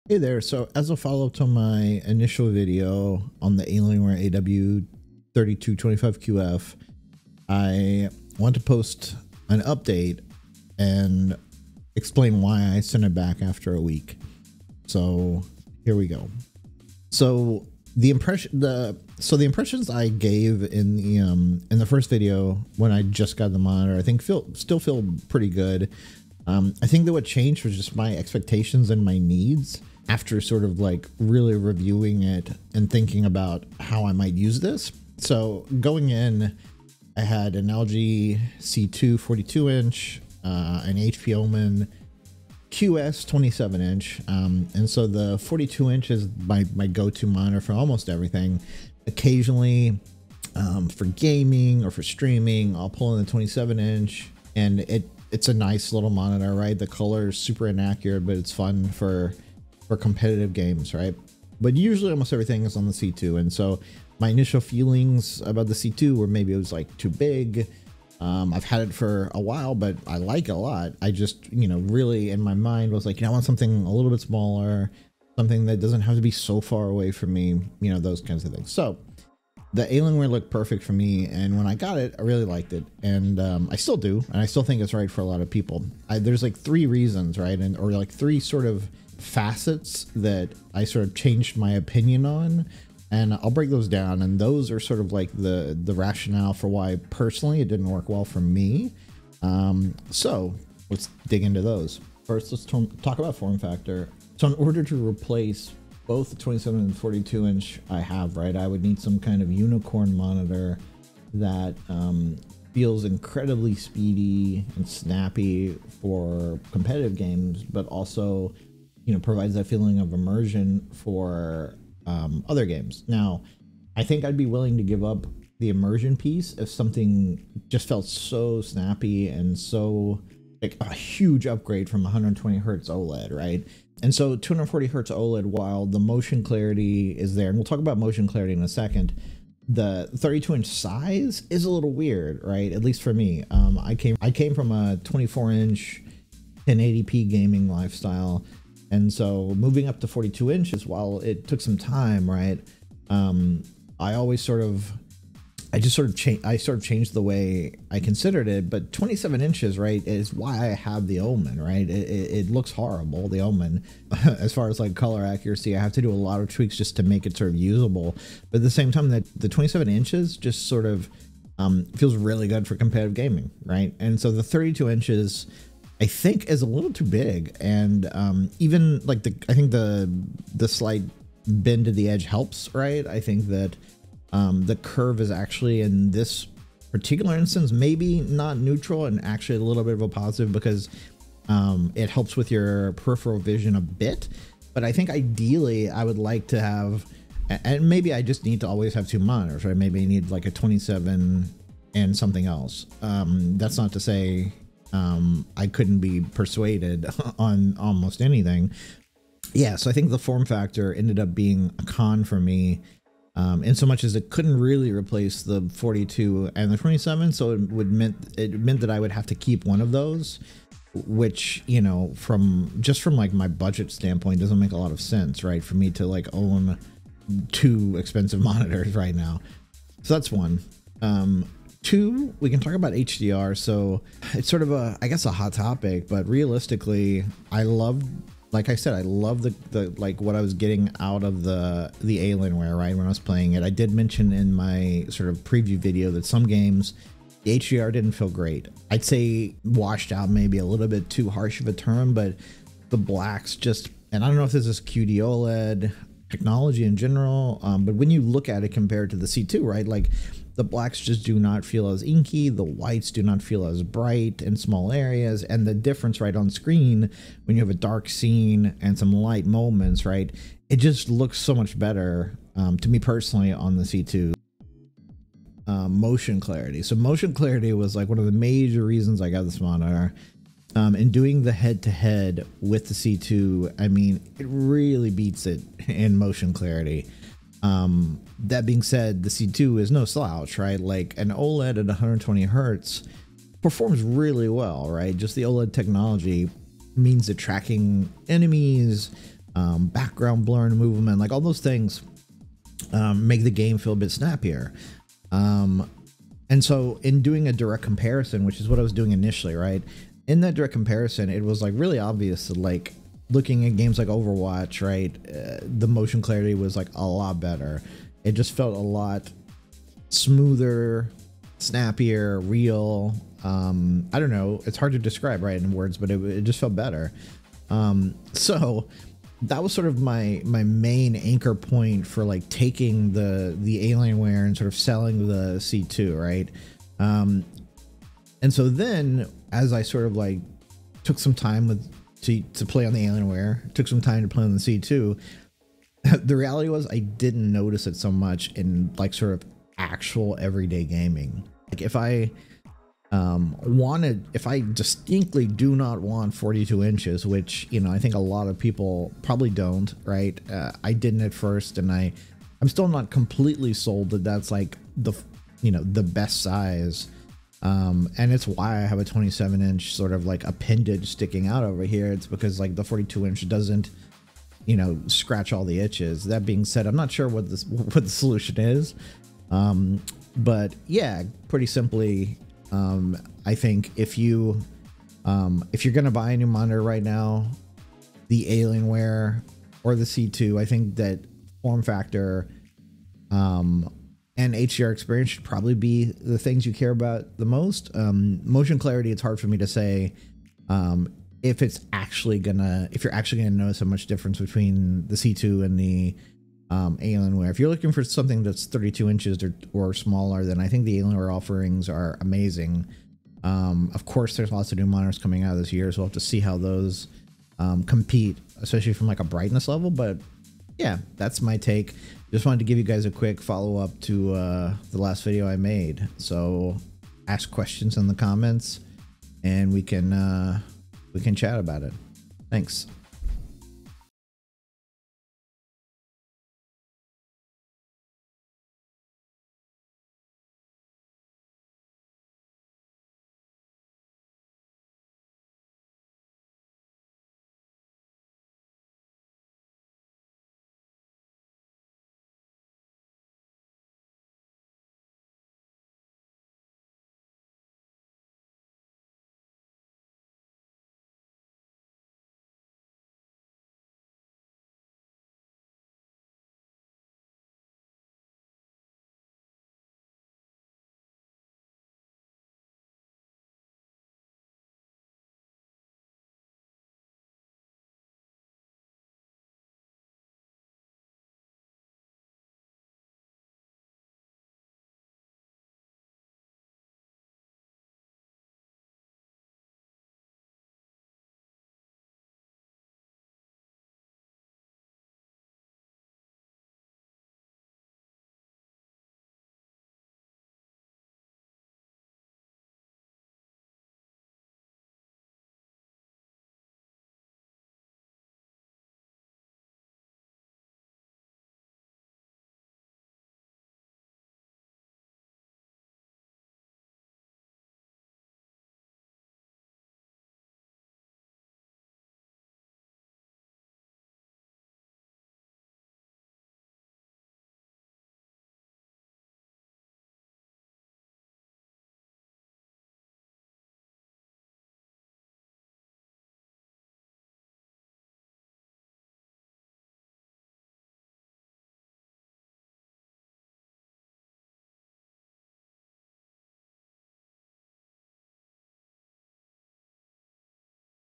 Why I returned the Alienware AW3225QF in favor of an LG C2
As a follow up to my initial review, I spent more time with the Alienware and, while I think it's a top-tier model and the right choice for most people, my specific needs and budget didn't make sense in keeping it.

I'll talk form factor, HDR, and Motion clarity.